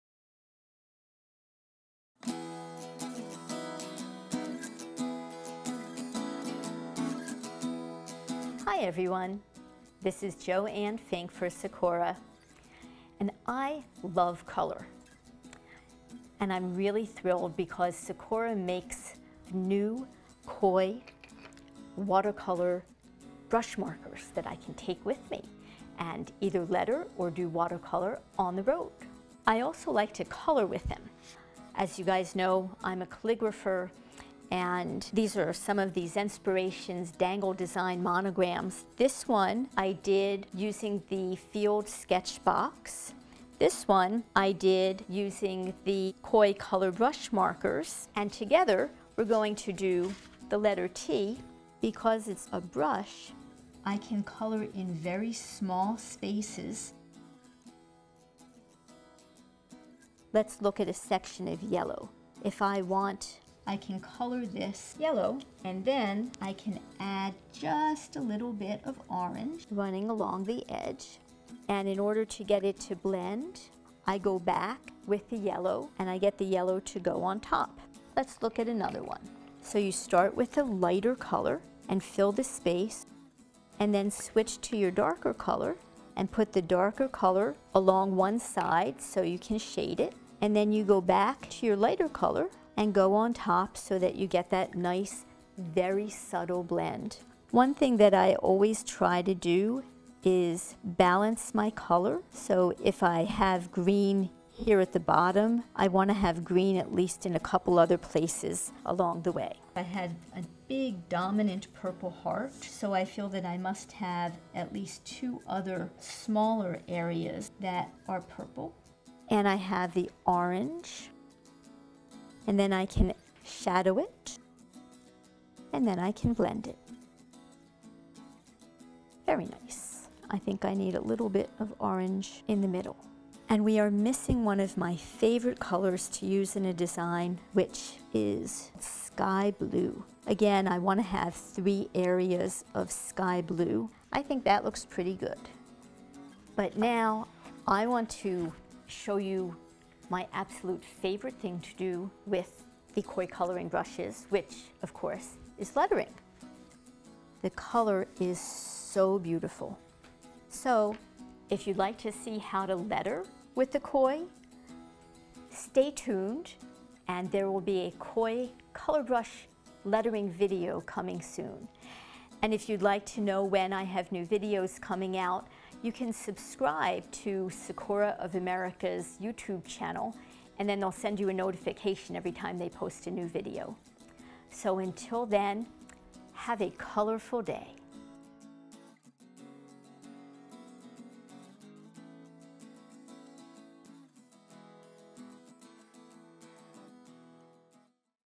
Coloring Zenspirations Patterned Monograms with Joanne Fink
In this video, Zenspirations founder and calligrapher Joanne Fink gives creative monogram coloring techniques using Koi Coloring Brush pens. Learn how to blend hues using these versatile, watercolor markers.